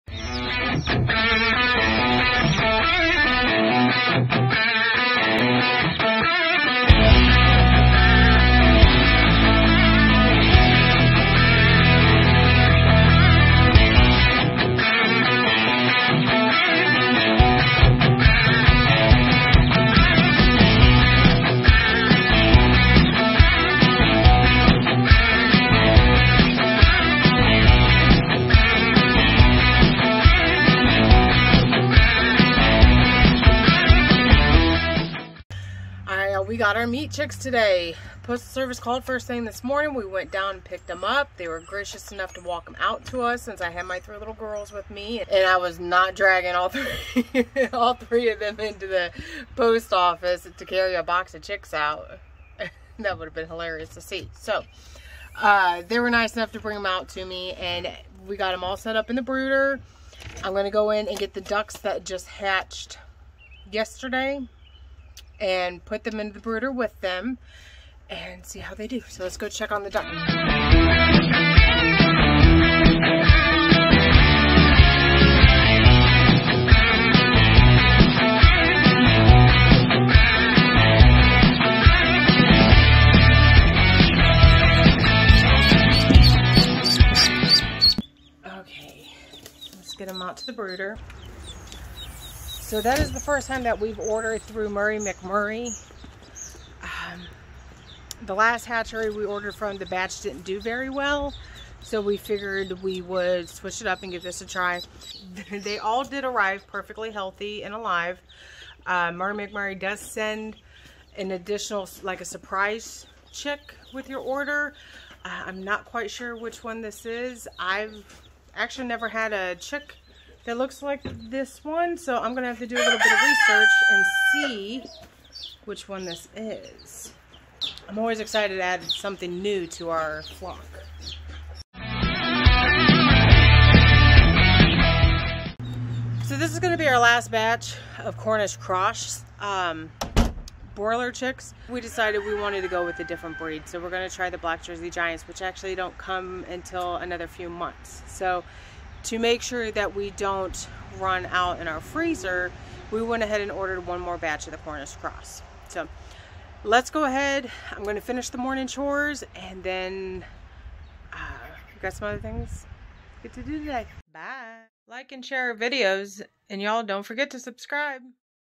Post office did what?
Setting up the brooder for broiler chicks.